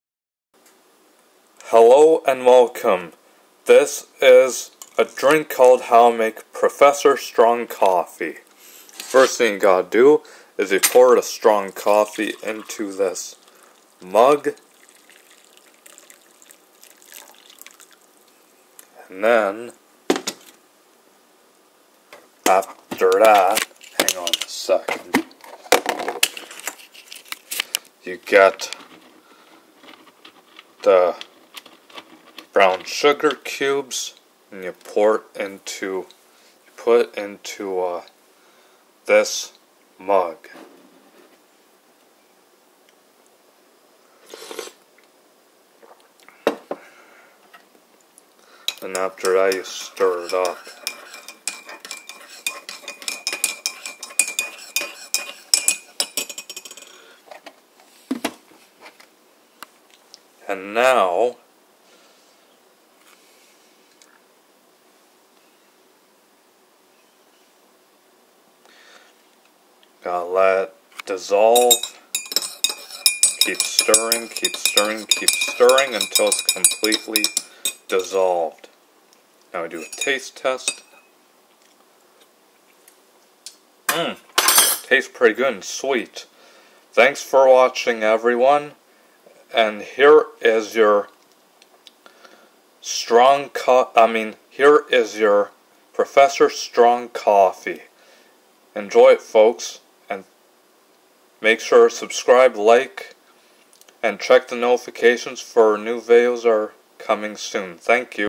How to Make Professor Strong Coffee
1. Pour 1 cup of Strong Coffee into a mug.
2. Put cane sugar cubes into the strong coffee & stir it up.
There's the Professor Strong Coffee folks, enjoy!